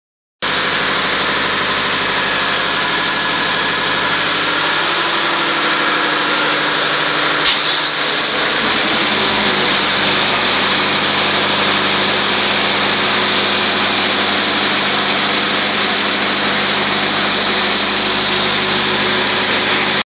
PT. Servewell Offshore - TB. Servewell Patriot 3
Maintenance test for Emergency Genset